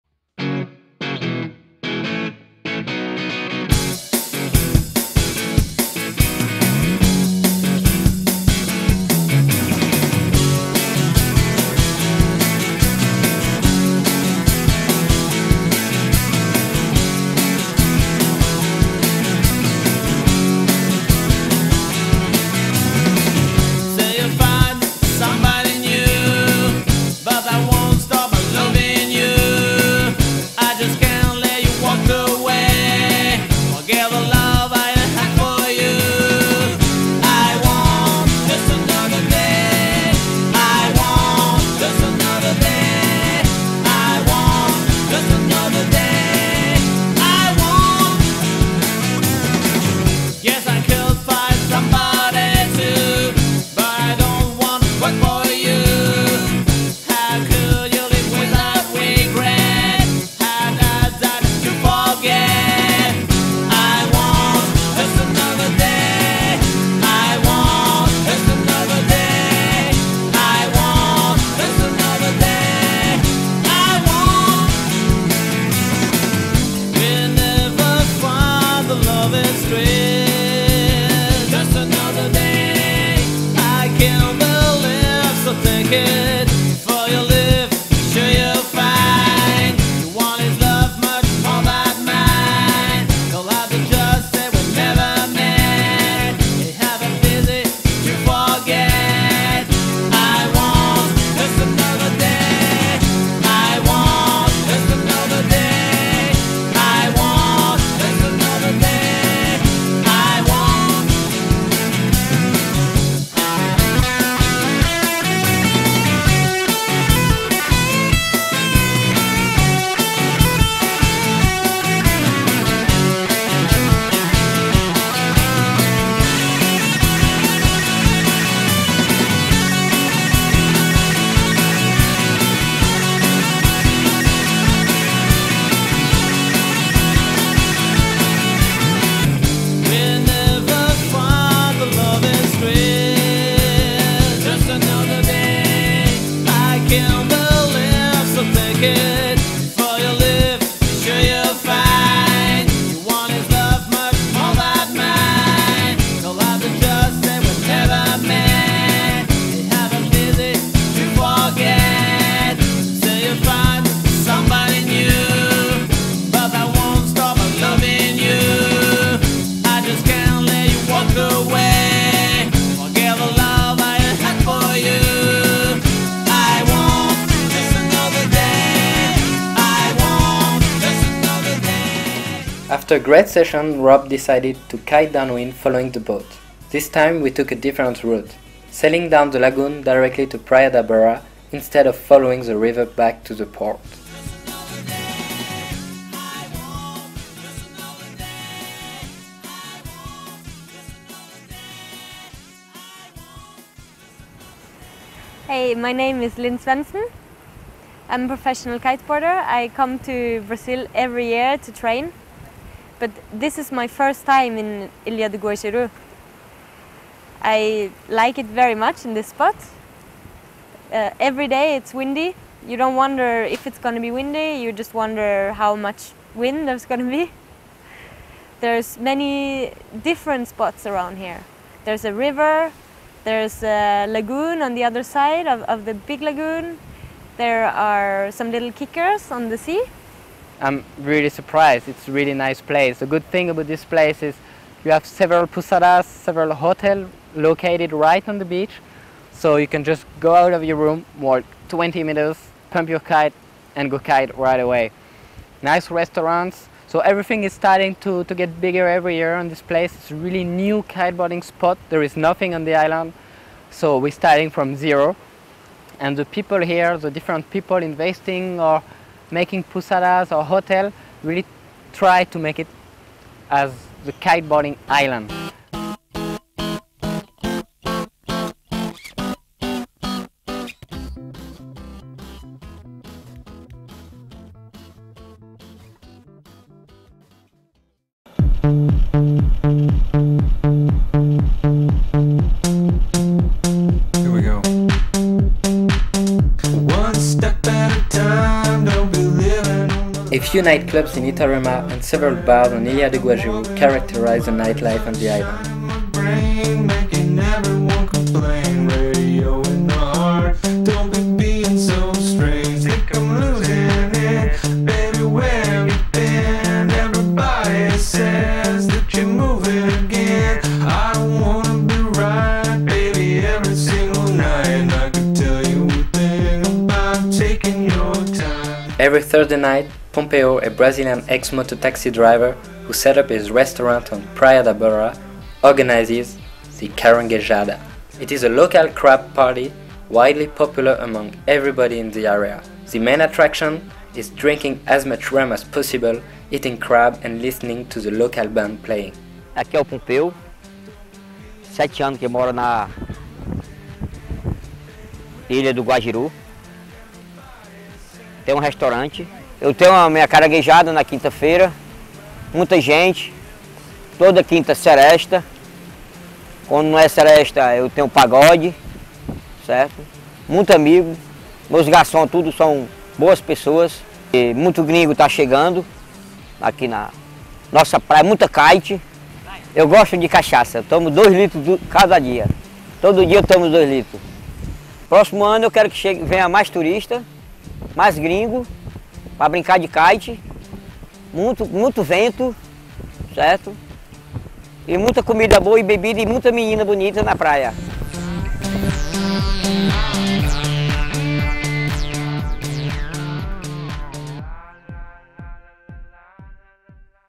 Part 3/4 - Discovering Ilha do Guajiru - Brazil- Jeremie Tronet kiteboarding DVD
Find here the fourth part of the DVD I made in Brazil end of 2008.
Different riders, different style of kitesurfing and windsurfing discovering a new kite spot in the north of Brazil, Ilha do Guajiru. We had fun making this movie, kiting every day in great condition and that's all it matters !
You can also find this DVD with your favorite kitesurf magazine worldwide !